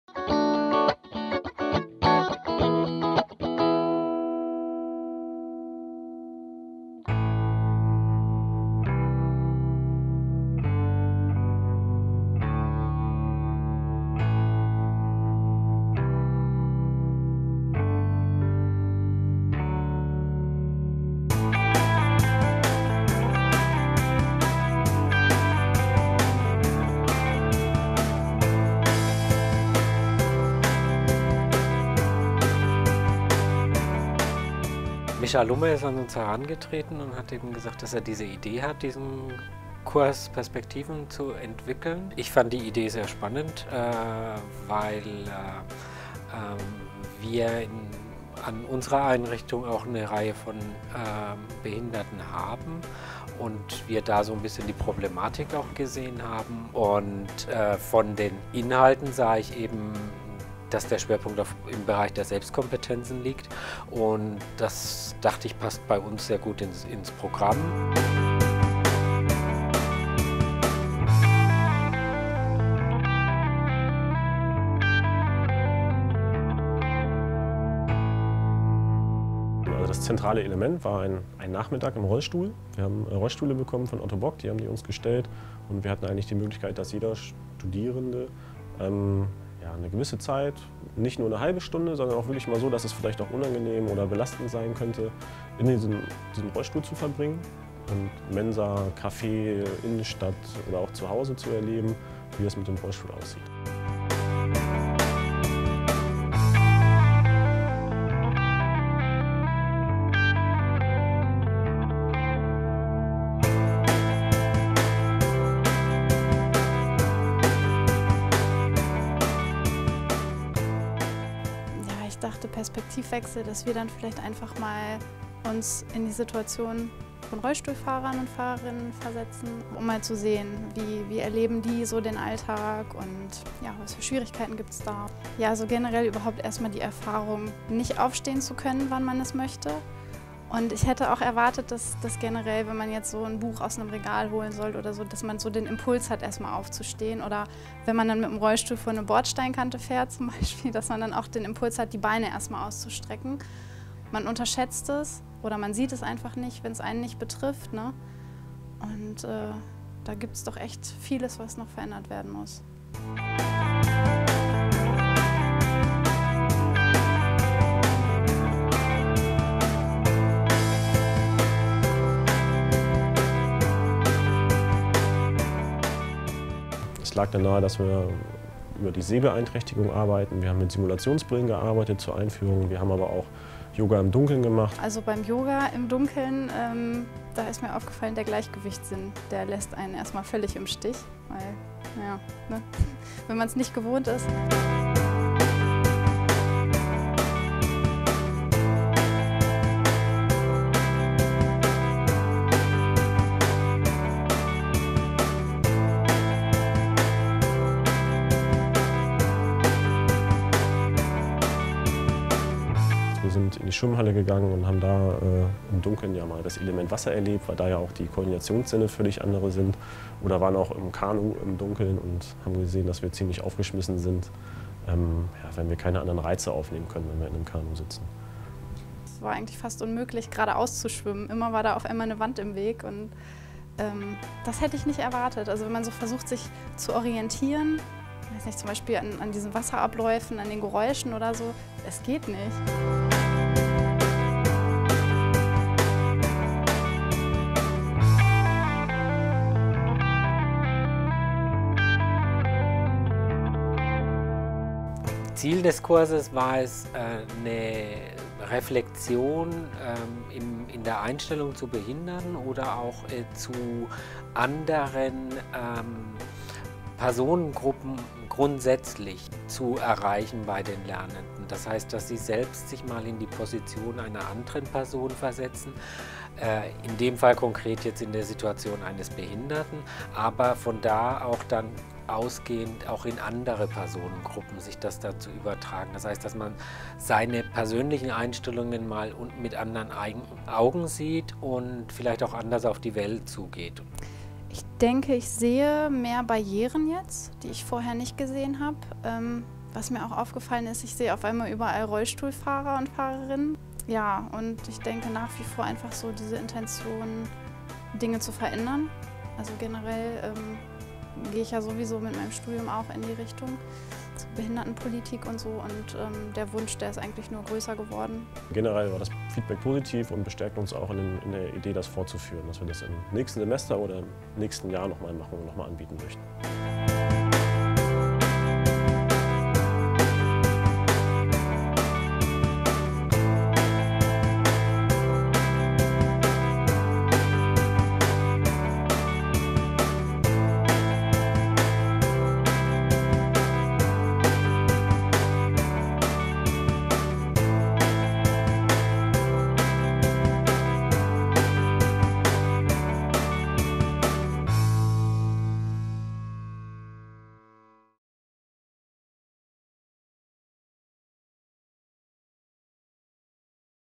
Perspektivenwechsel
Mit Unterstützung des Bereichs Ideenmanagement der Abteilung Studium und Lehre wurde in Zusammenarbeit des Hochschulsports und der ZESS im Wintersemester 2013/14 erstmalig der Kurs "Perspektivenwechsel: Studieren unter körperlicher Beeinträchtigung - Ein Tag im Rollstuhl" angeboten. Aufgrund der sehr positiven Resonanz wird dieses Seminar auch in den kommenden Semestern an der ZESS angeboten. Univision hat den Kurs mit der Kamera begleitet.